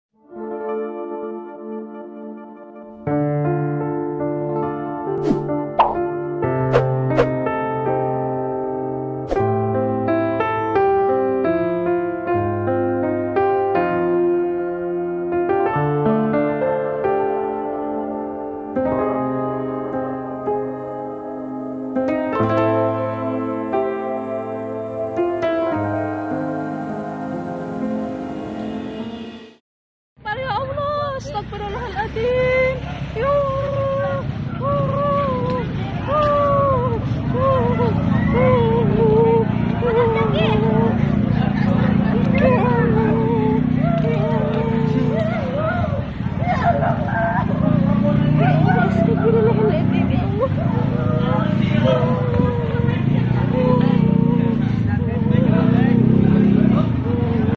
Rem Truk Blong Tabrak Warga Sedang Ngabuburit di Bumiayu Brebes
Kecelakaan maut terjadi di kecamatan Bumiayu Brebes, Minggu (20/5) pukul 16.30 WIB . Kecelakaan itu disebabka oleh truk yang mengalami rem blong.